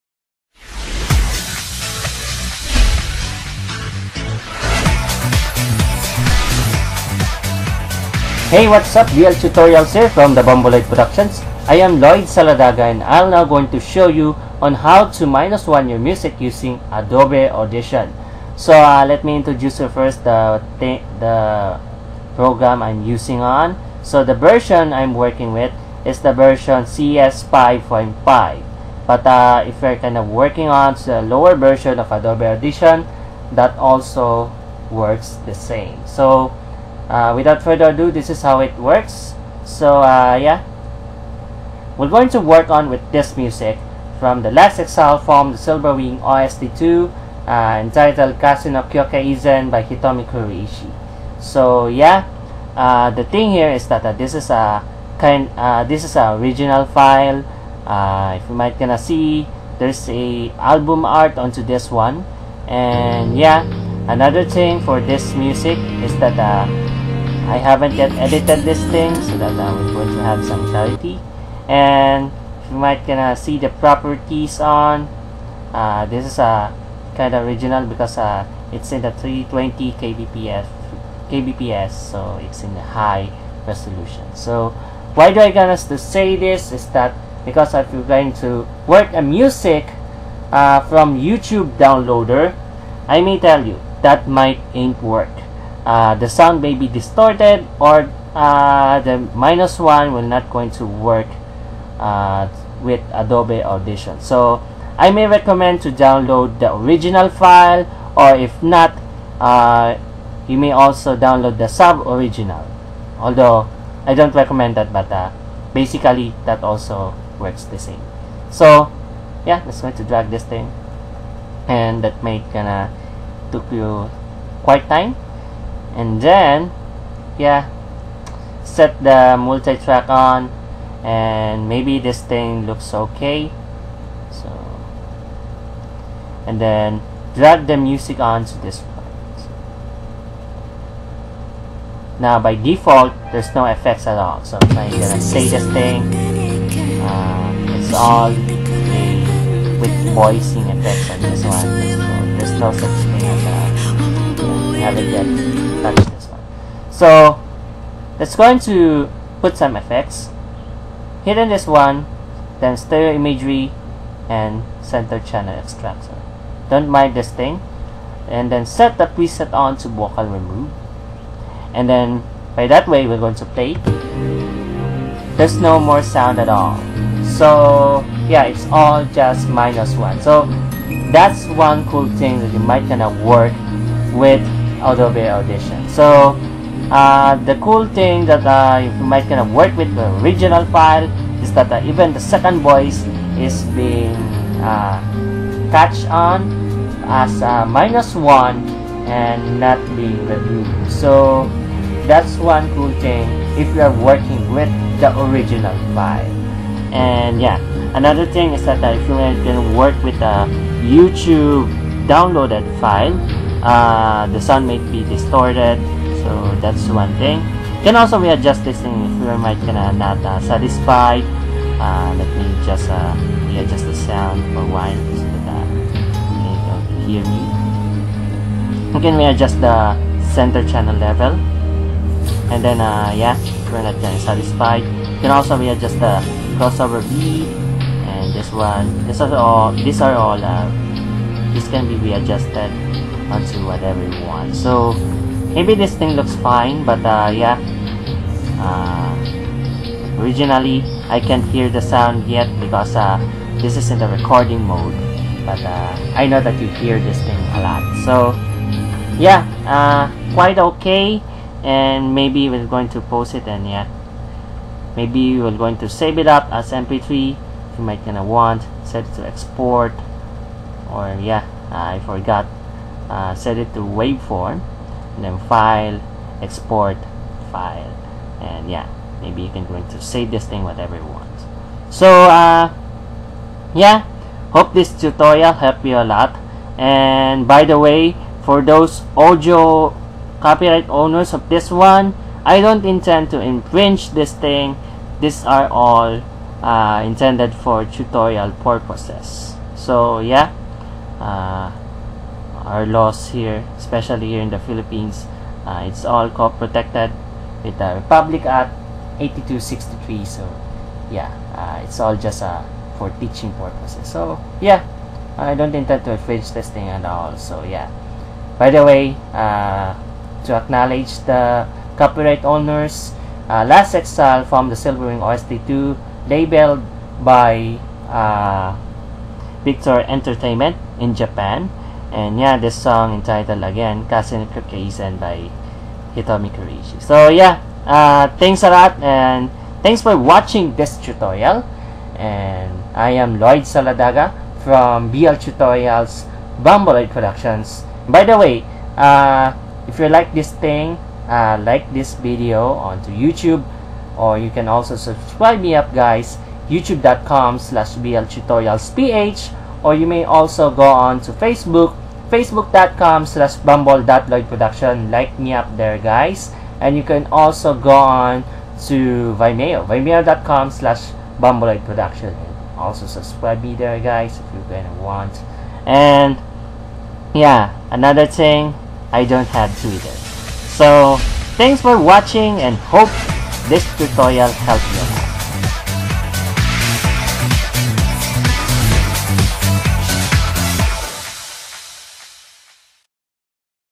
BL TUTORIALS 02: HOW TO REMOVE VOCALS OF A SONG (Adobe Audition CS5)
How to remove vocals of a song (minus-one)
works with ADOBE AUDITION CS5.5 and its predecessors.